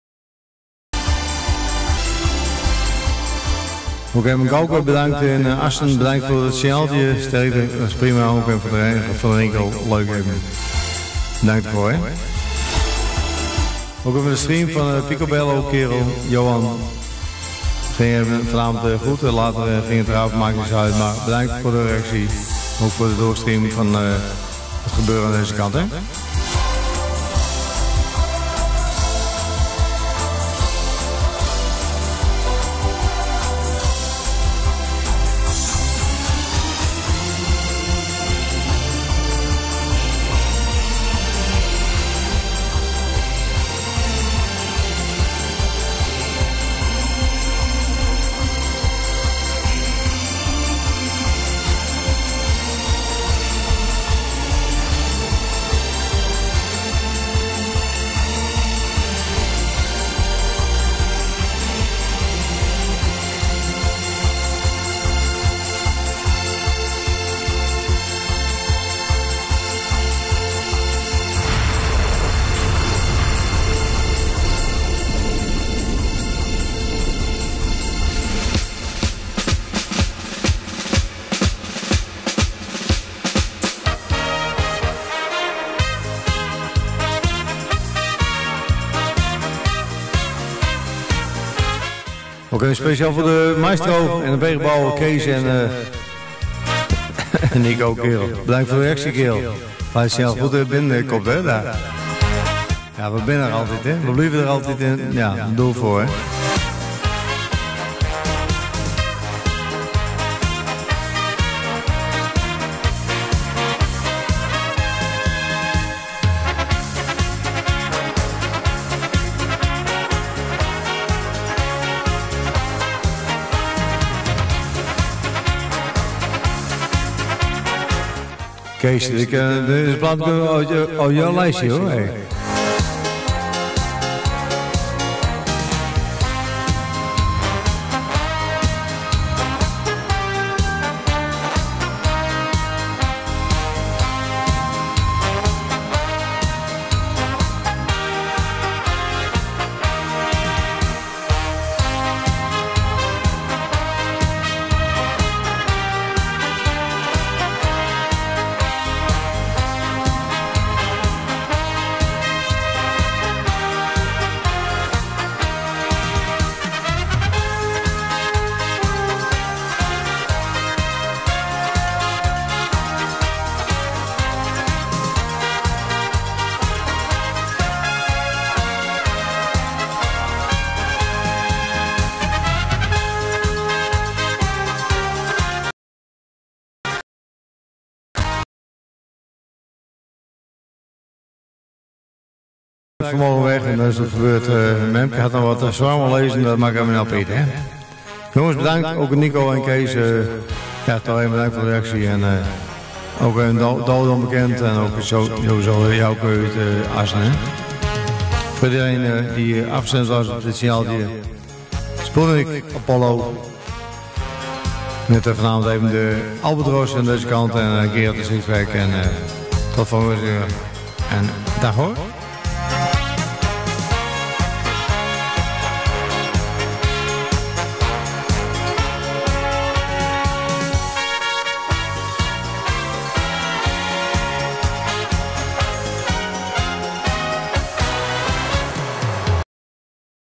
PERSEUS FM+ DX SCHILDWOLDE:Apollo en de Sixpack [DUTCH PIRATE STATION]
received on a micro telecom Perseus FM + and a 5 element yagi rotated at 710inch

RX Location: Schildwolde
Date: 16/11/2014